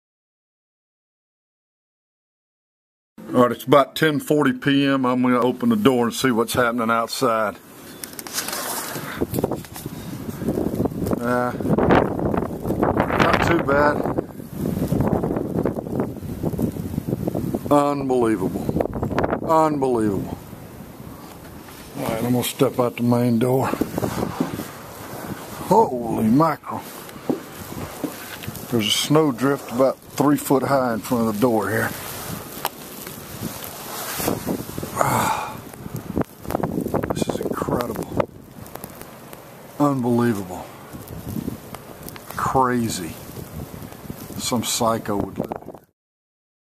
Chicago Blizzard 2011 - Part II
Taken in the Rosemont area of Chicago on Wed, 2/1/11 in the middle of the Blizzard